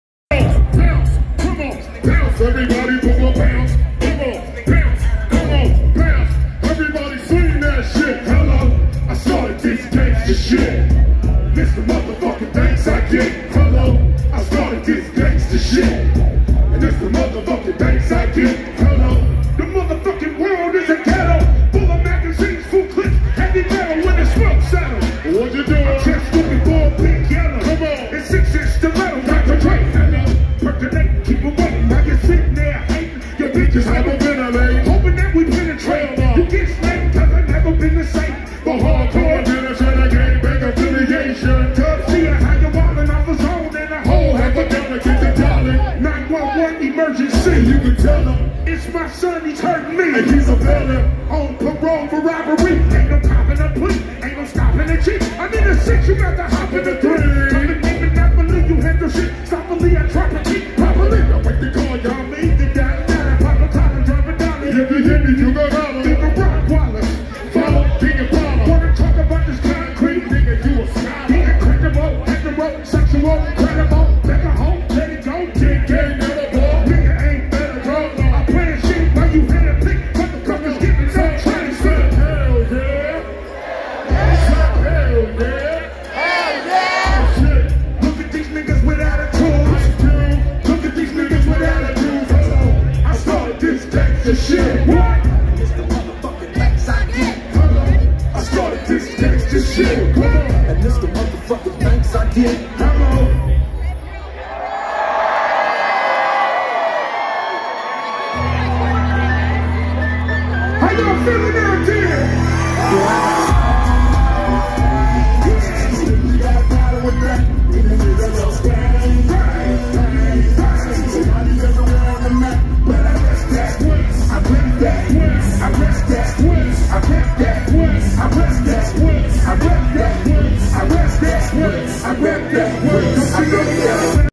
Ice Cube Concert, West Fargo, ND Fri, Aug 30 2024
Intro to the ICe cubw concert